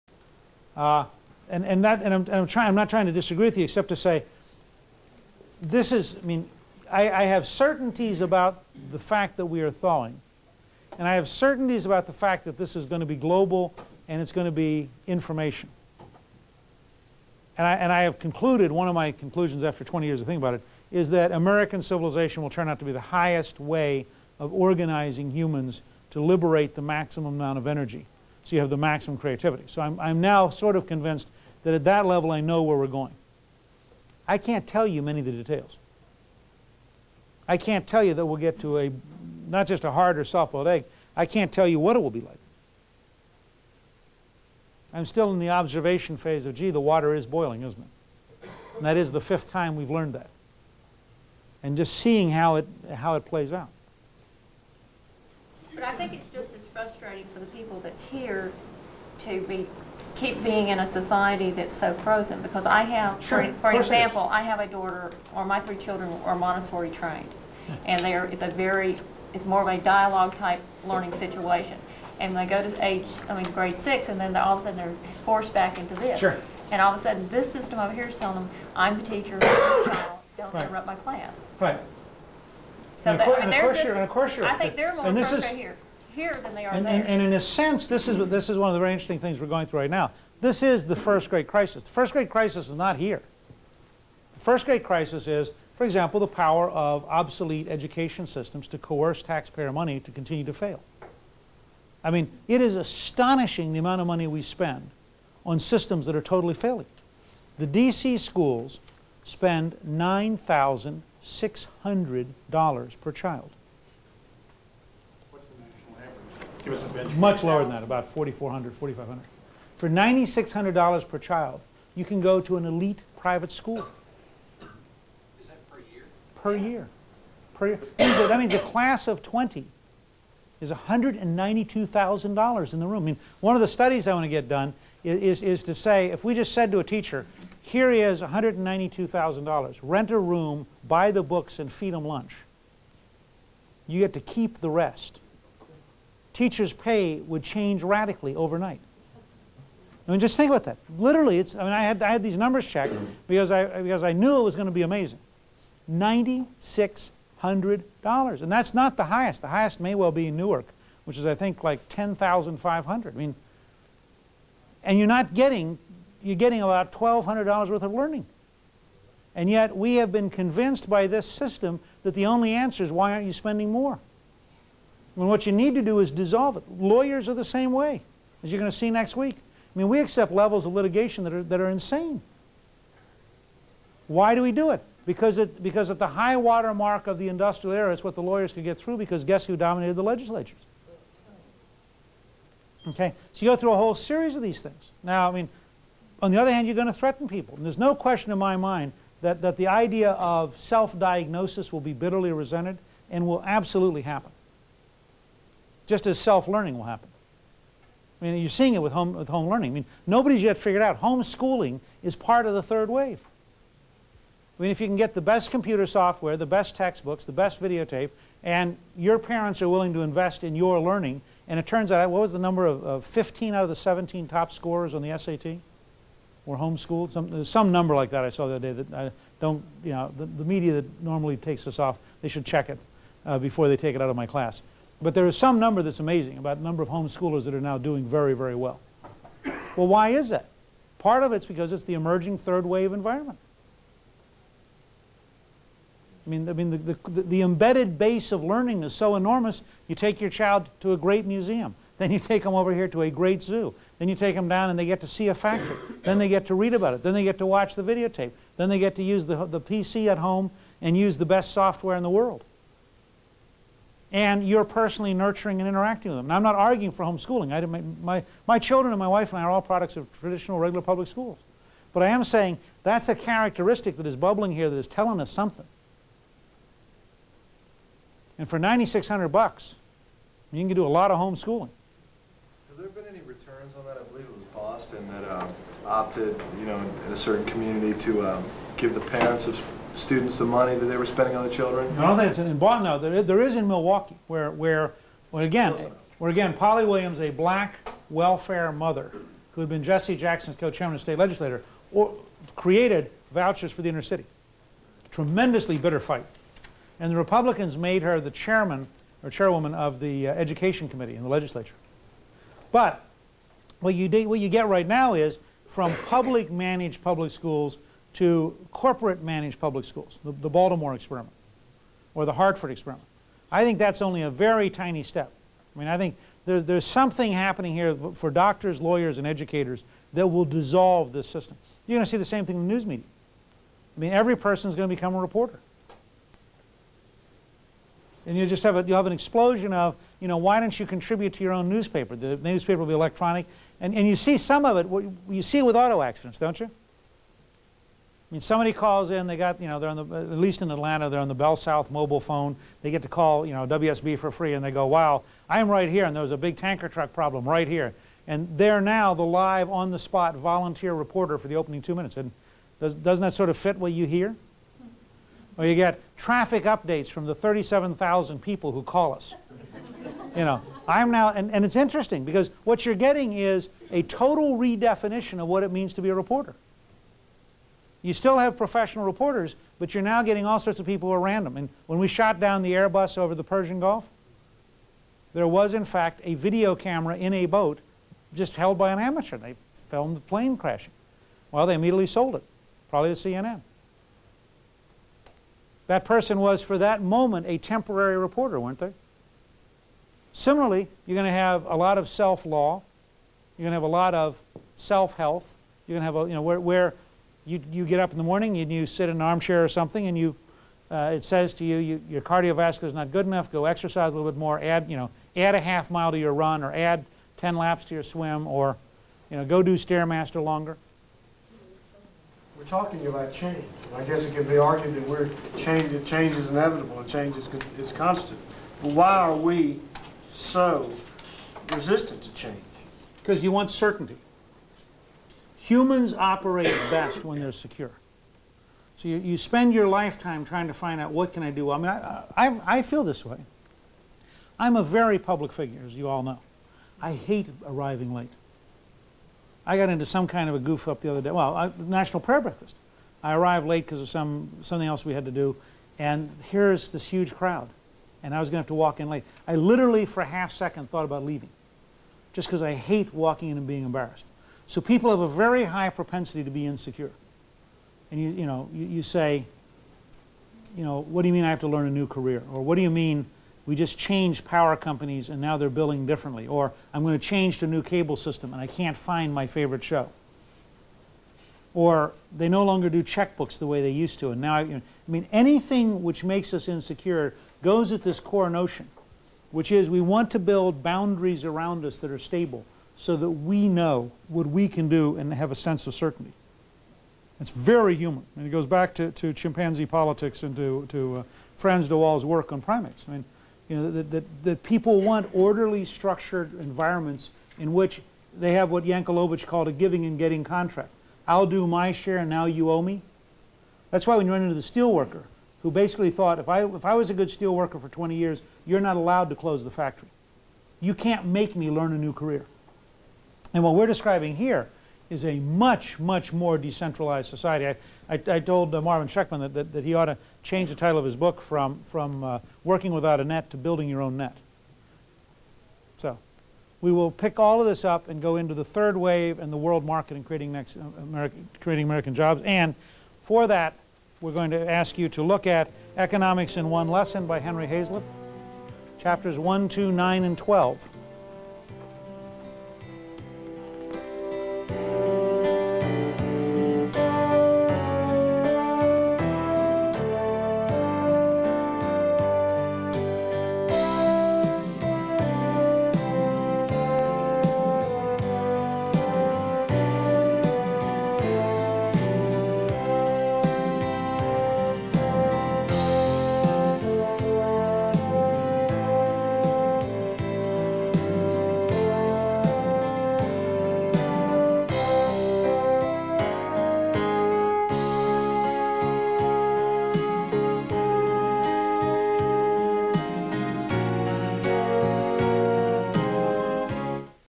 RAC07n.mpg
Renewing American Civilization Course - Lecture Seven -- February 18, 1995 -- The Third Wave and American Civilization (Part 12 of 12)